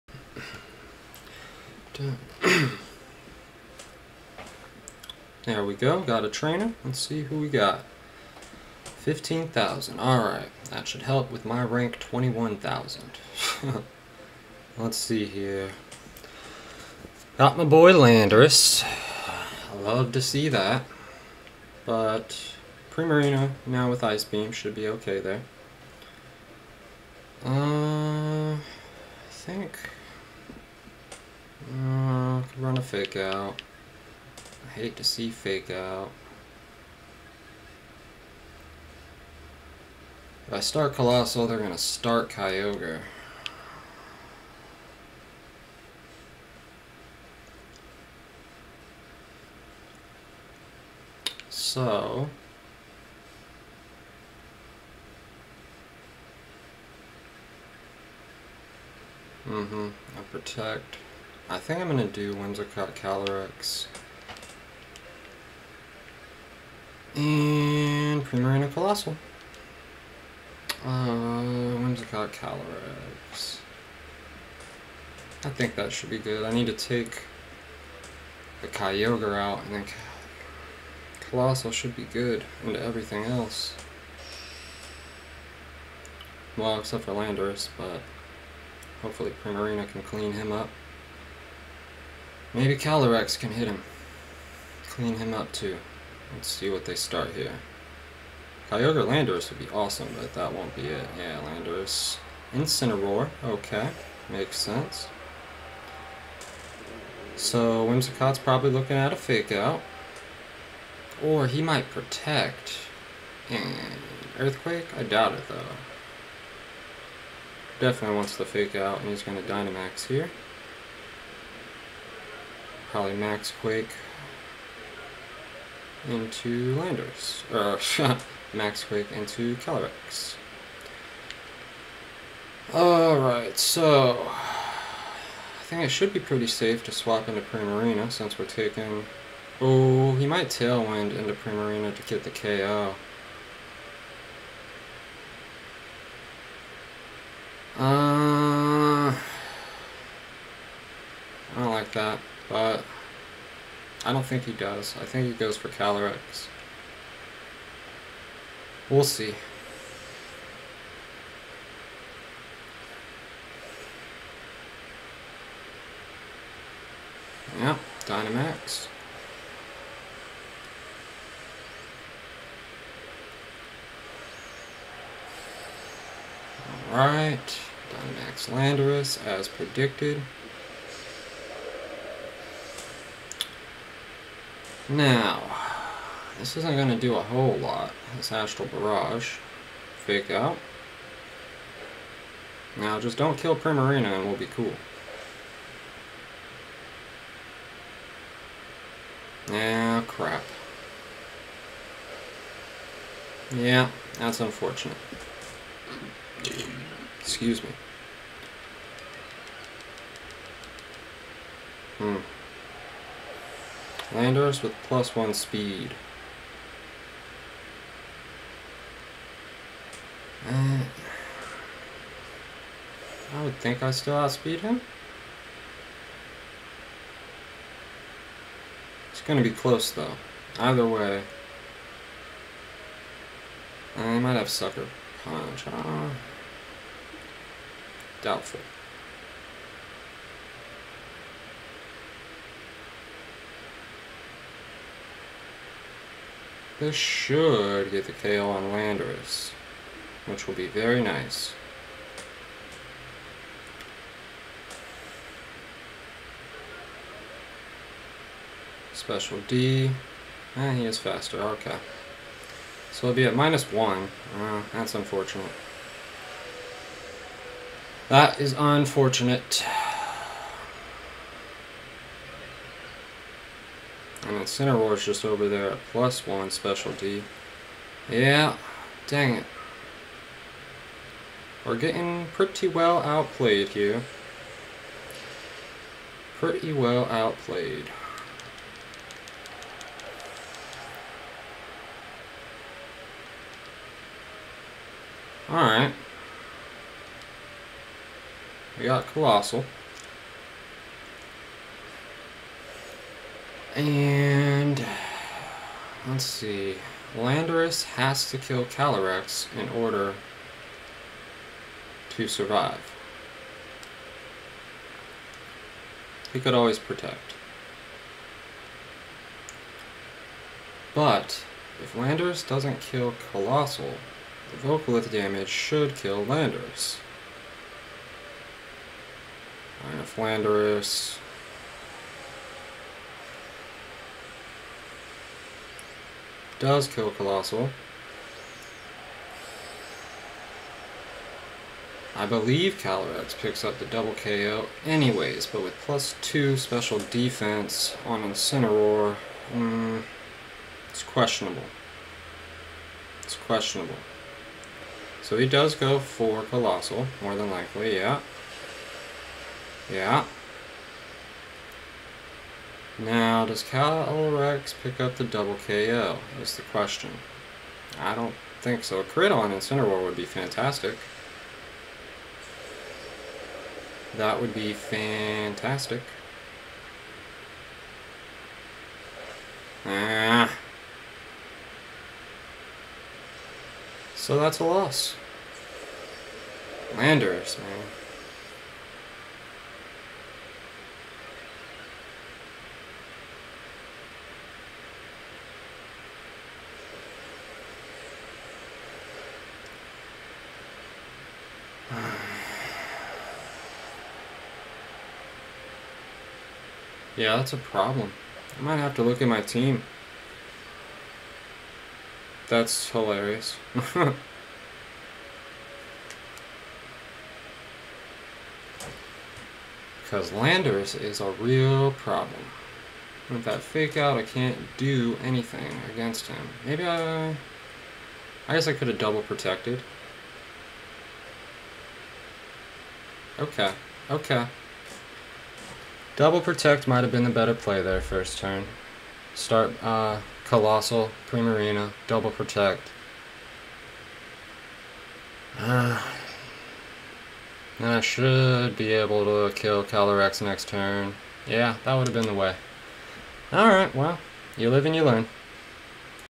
NitemaRe 2/28
Single battle with NitemaRe from 2/28 showcasing the ladder grind.